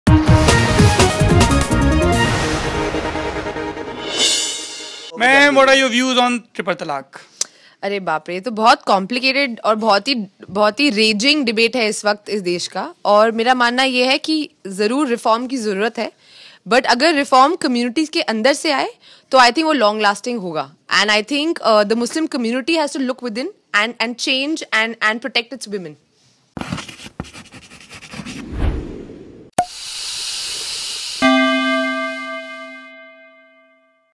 ट्रिपल तलाक़ पर क्या बोलीं हुमा कुरैशी | The Lallantop
Huma Qureshi talks about tripple talaq
Produced By: The Lallantop
Edited By: Amitesh Sinha